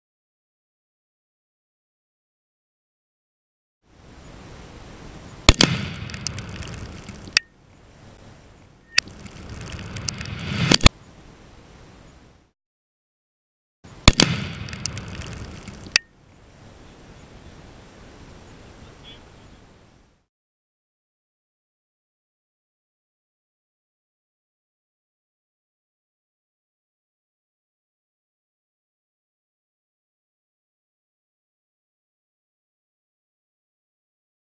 1/2lb tannerite blows up some bananas and hot dogs in slow motion
Just fooling around with a small bit of Tannerite and a gopro.